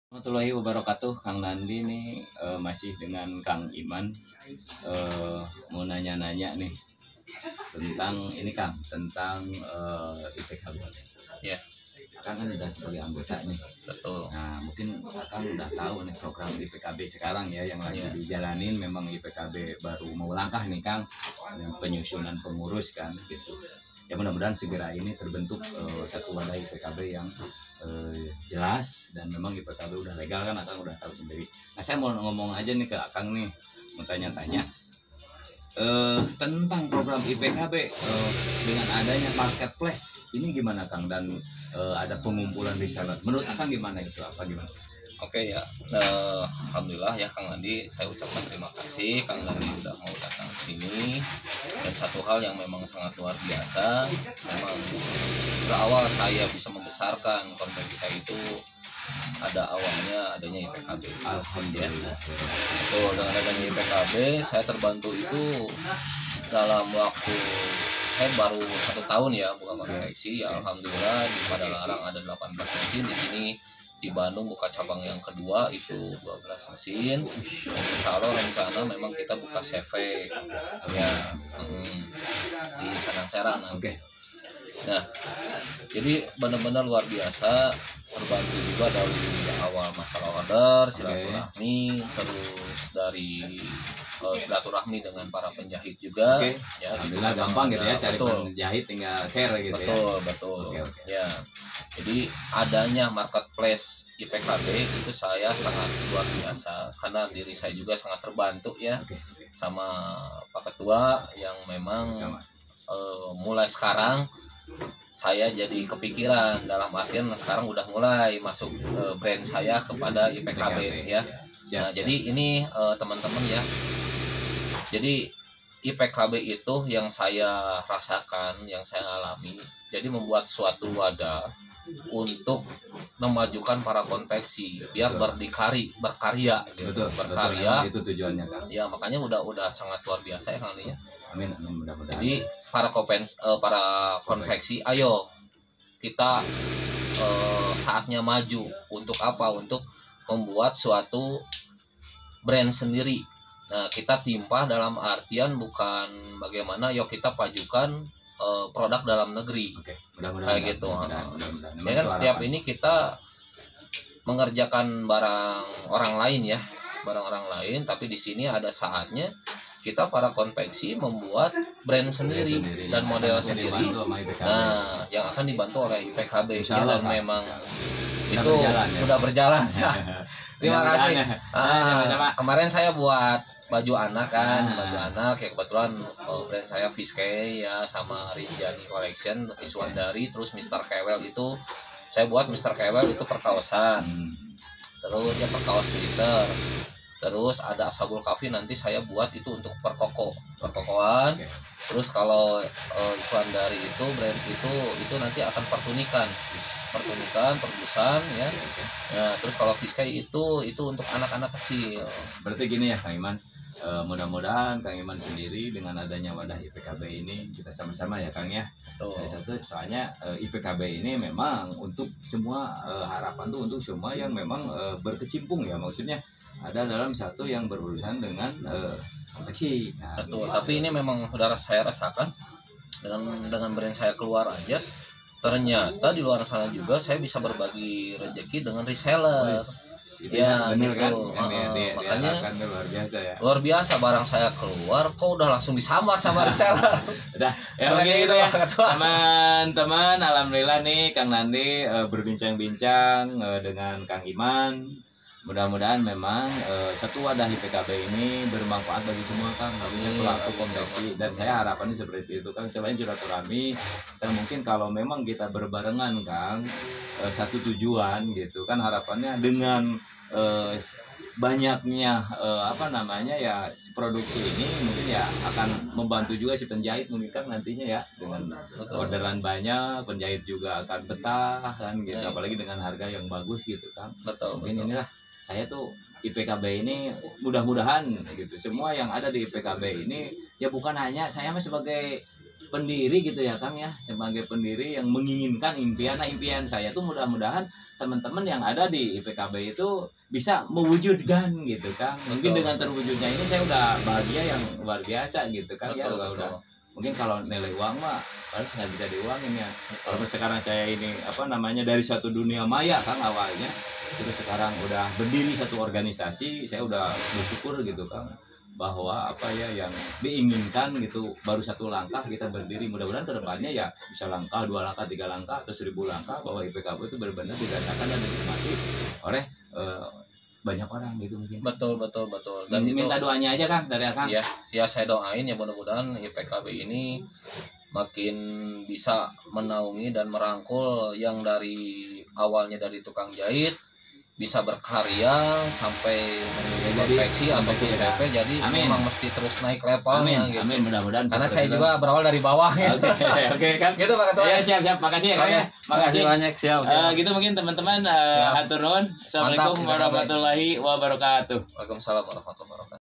Konveksi Berkembang Setelah masuk ke Organisasi IPKB
konveksi  menerima  CMT FOB
spesialis kaos dan kemeja